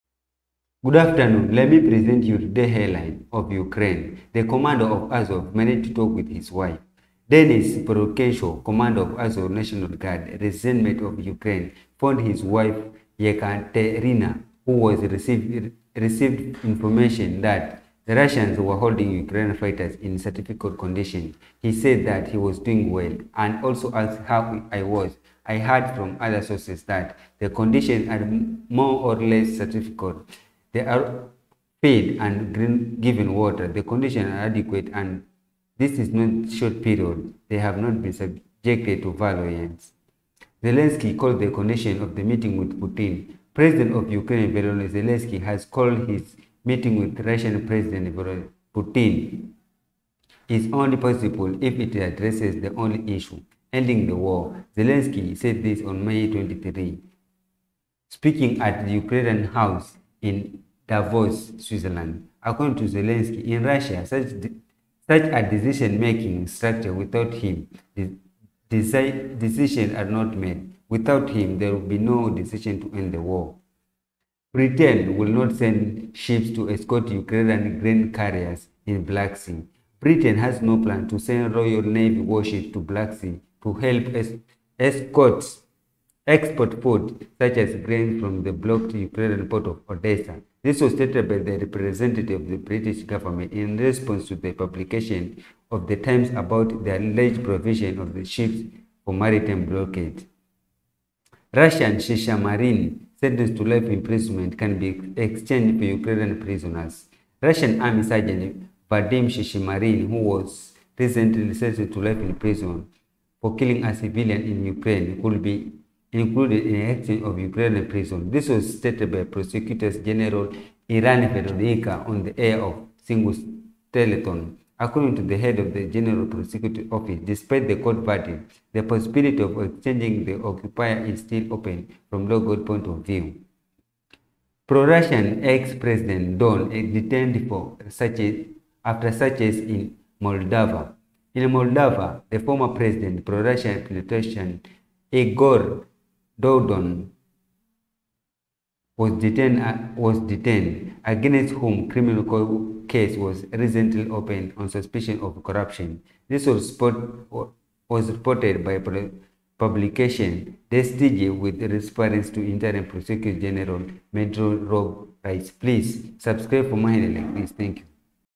Ukraine Headlines Today | The commander of "Azov" managed to talk with his wife.
Ukraine Headlines Today
1:The commander of "Azov" managed to talk with his wife.
2:Zelensky called the condition of the meeting with Putin.
3:Britain will not send ships to escort Ukrainian grain carriers in the Black Sea
4:Russian Shishimarin sentenced to life imprisonment can be exchanged for Ukrainian prisoners
5:Pro-Russian ex-president Dodon detained after searches in Moldova